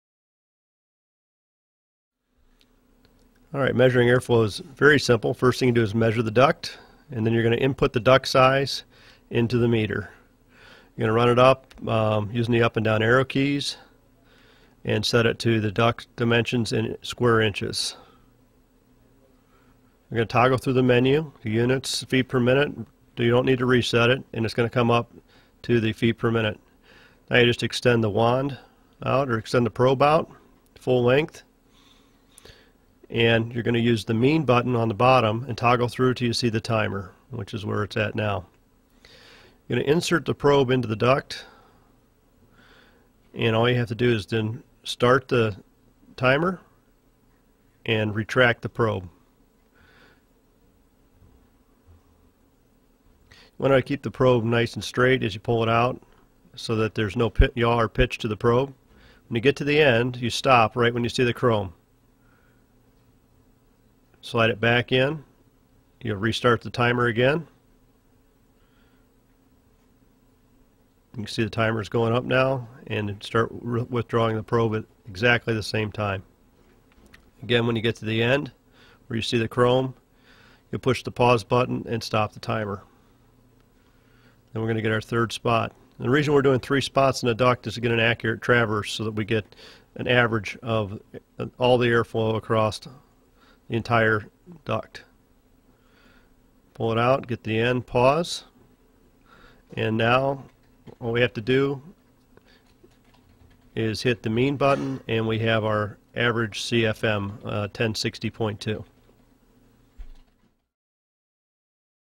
Testo Vane Anemometer 416
The testo 416 is a compact anemometer with a fixed vane probe. The measuring instrument is particularly suitable for flow velocity measurements in ducts thanks to the
extendable telescope (max. length 890 mm, diameter 16 mm). The volume flow is directly shown in the display. For volume flow calculation purposes, the duct crosssection area can be conveniently entered into the testo 416.
The timed and point mean value calculations provide information on the average volume flow. The Hold function allows the current measurement value to be fixed in the display. Min./max. values are also displayed at the press of a button. The optional TopSafe reliably protects the instrument from dirt and impact, ensuring a particularly long working life.